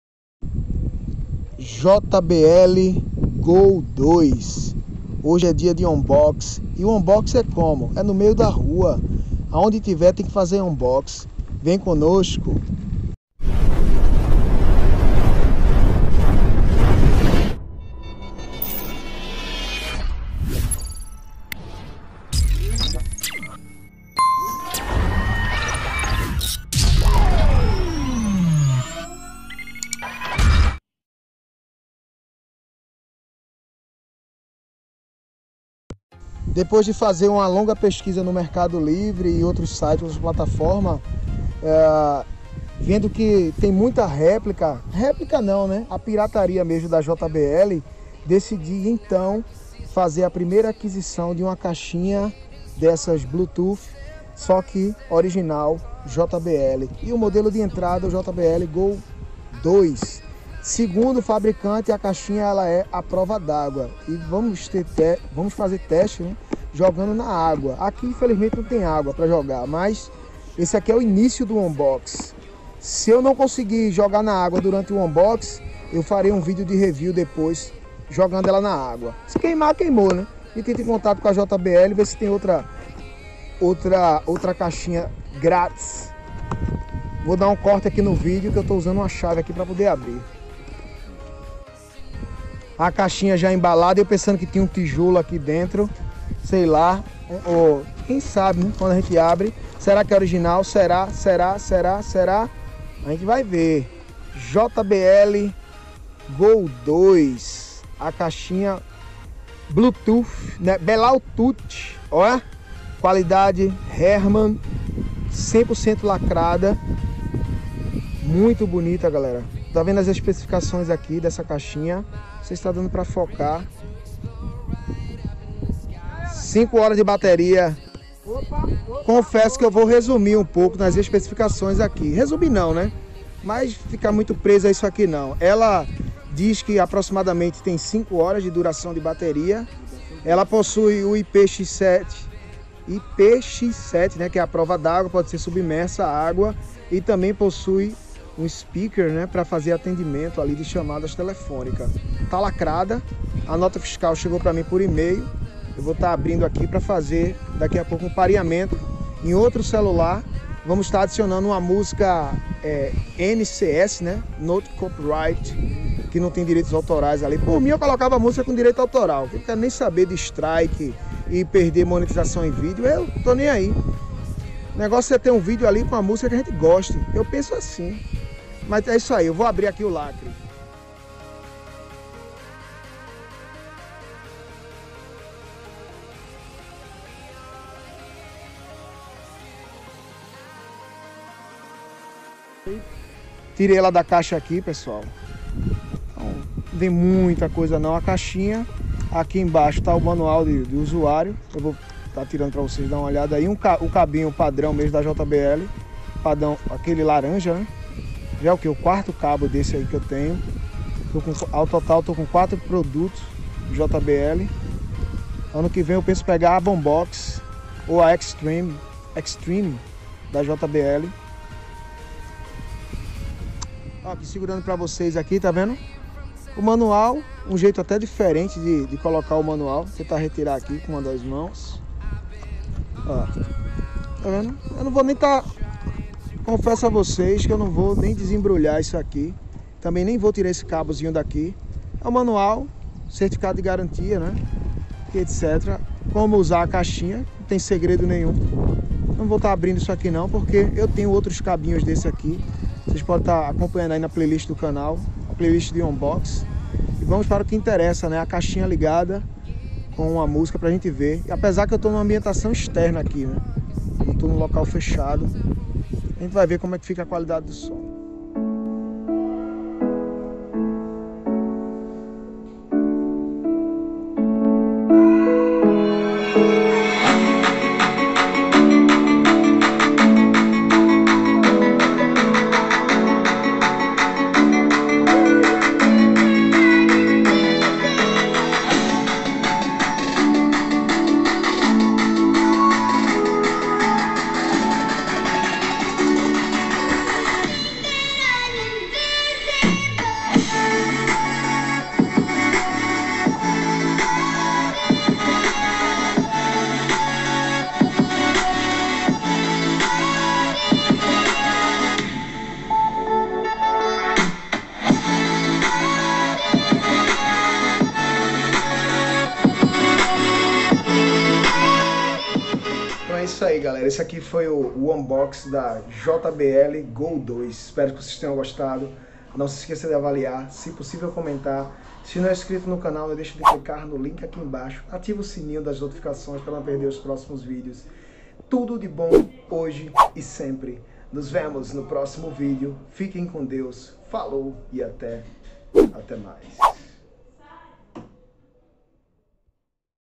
Unboxing JBL GO2
Neste vídeo eu faço o Unboxing do JBL Go2 original